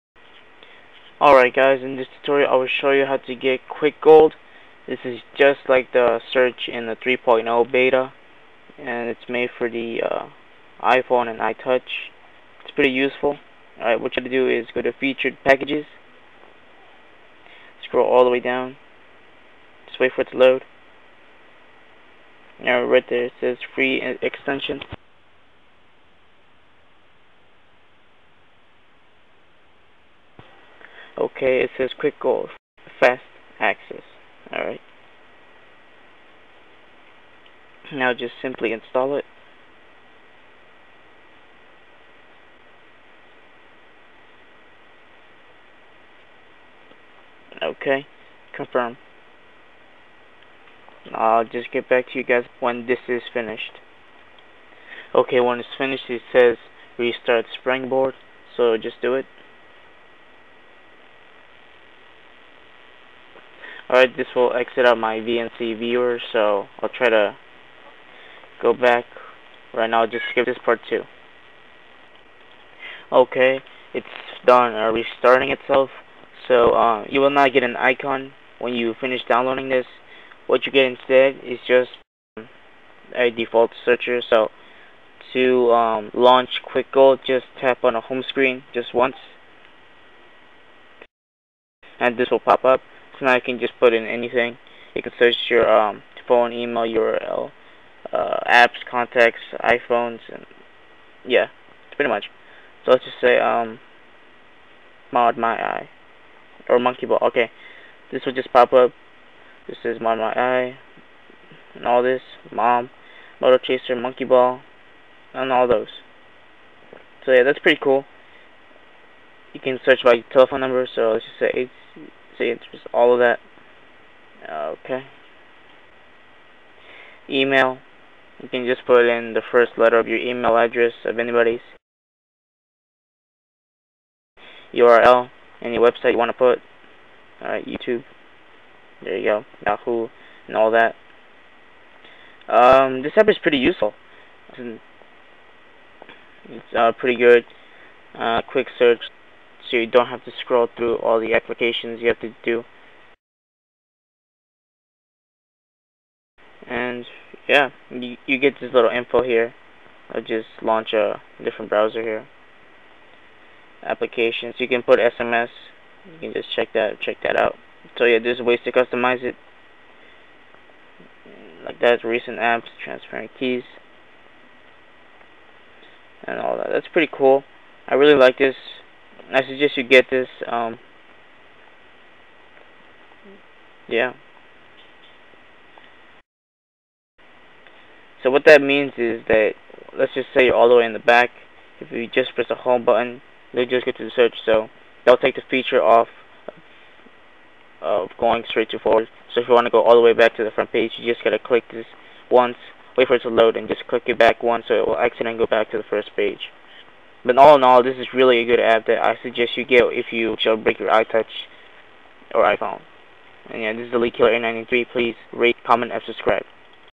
How to get QuickGold [Fast Access Search] on your iPhone and iTouch
In this video i will show you how to get QuickGold, an Fast Access Search which is like the search feature in the 3.0 beta
1) Go to Cydia
2) Go to Featured Packages
3) Scroll Down and tap on QuickGold
4) Install
5) NOTE: Its not an App
6) Press the Home Button and begin your search